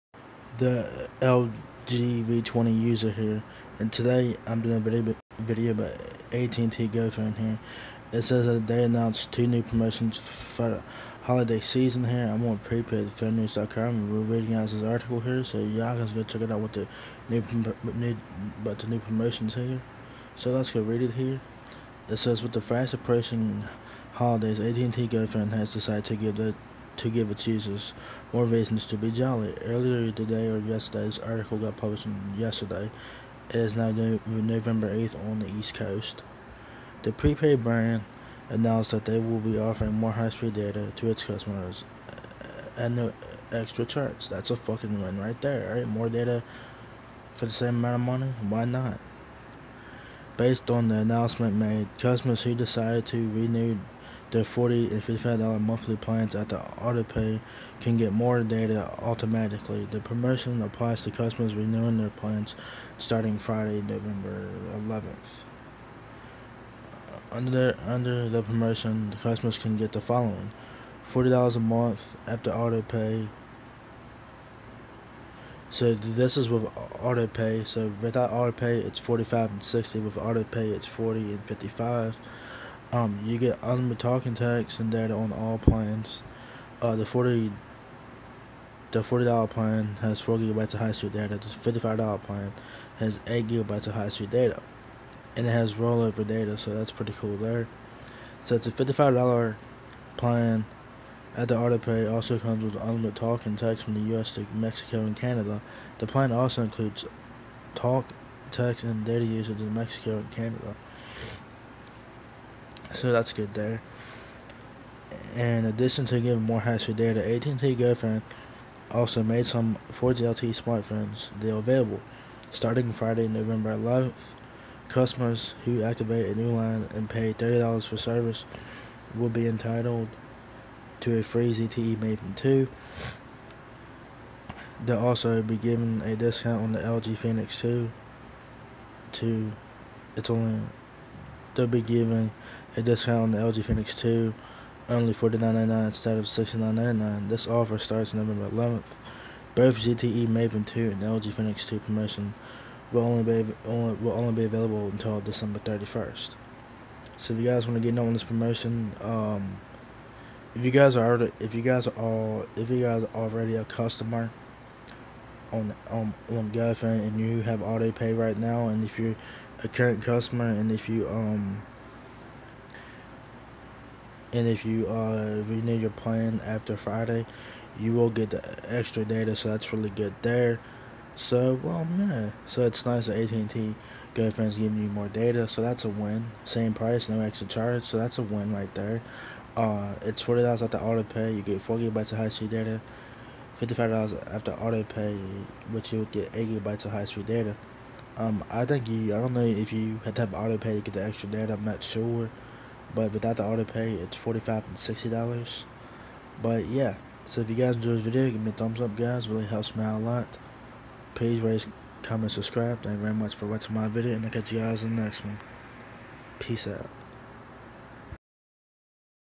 AT&T GoPhone Announced Two New Promotions For The Holiday Season
$ 40 Monthly Plan (after AutoPay) - unlimited talk and text, 4GB of high-speed data, and rollover data

 $55 Monthly Plan (after AutoPay) - unlimited talk and text, 8GB of high-speed data, and rollover data

The $55 monthly plan (after AutoPay) also comes with unlimited talk and text from the U.S. to Mexico and Canada. The plan also includes talk, text and data usage in Mexico and Canada.

ZTE Maven 2 - free
LG Phoenix 2 - $49.99